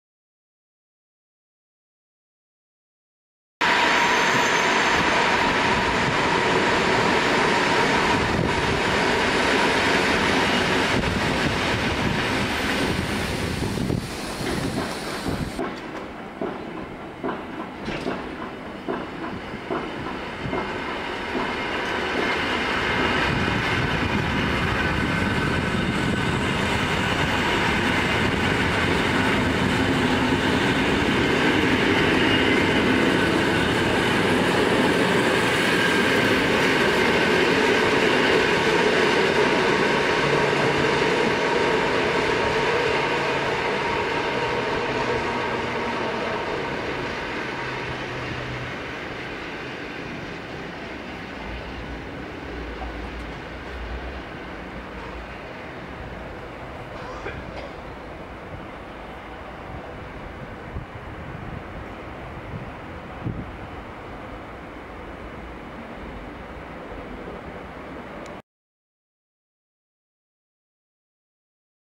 EN57-2013 Kraków Zabłocie jako pociąg osobowy PR relacji Rzeszów Główny - Katowice
EN57-2013

W trakcie postoju na przystanku Kraków Zabłocie jako pociąg osobowy Przewozów Regionalnych nr R34802 relacji Rzeszów Główny - Katowice.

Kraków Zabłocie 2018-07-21
kolej.krakow.pl